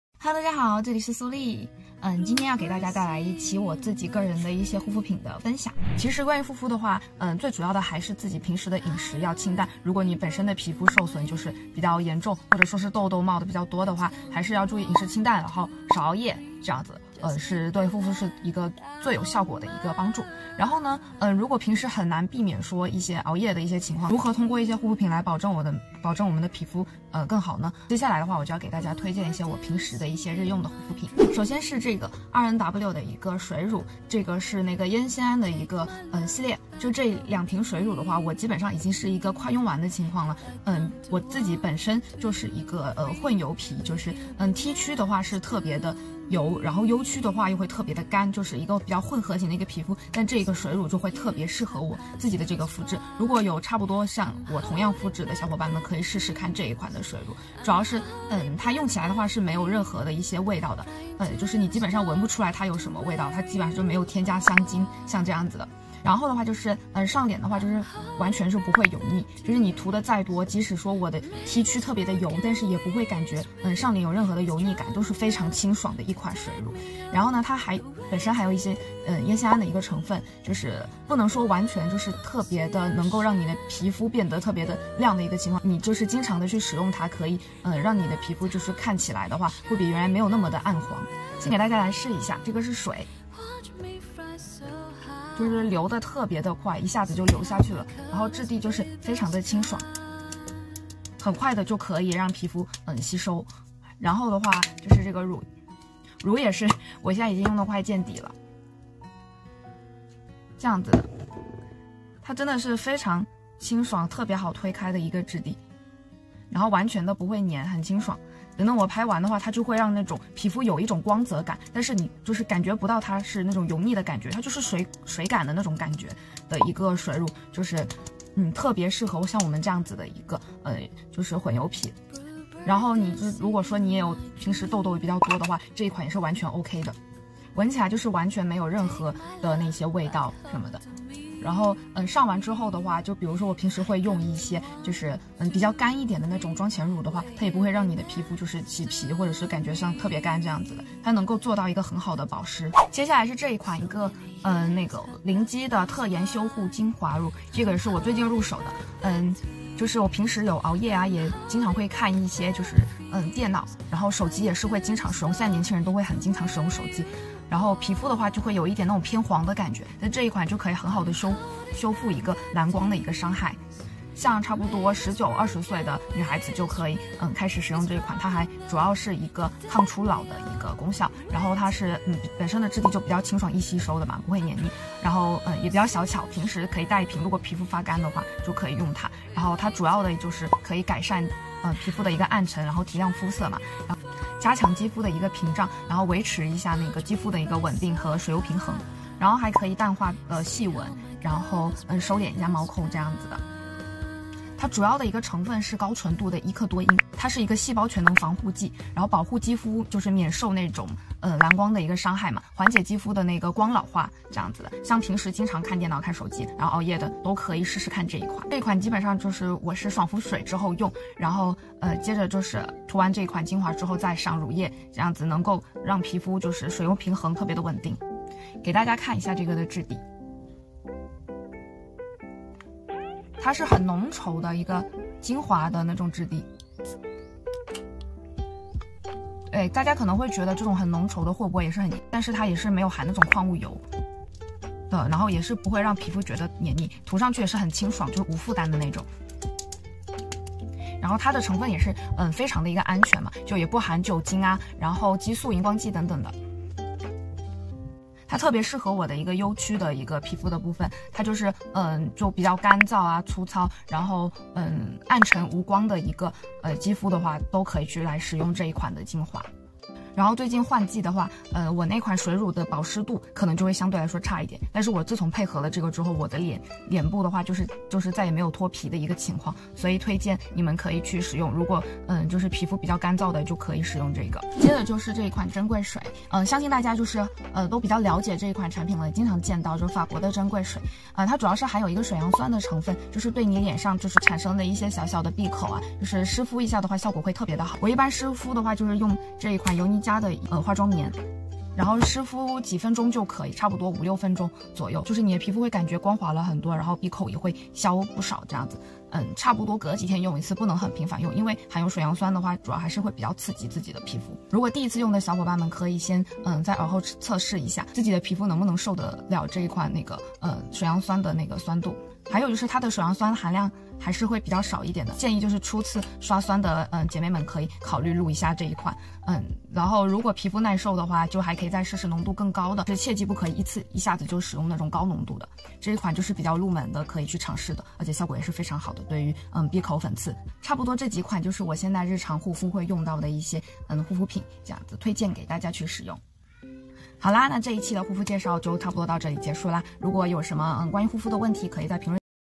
秋冬换季 难搞的混油皮有哪些平价又好用的护肤品？？笠笠的私藏护肤品 拒绝干燥脱皮敏感
嘿嘿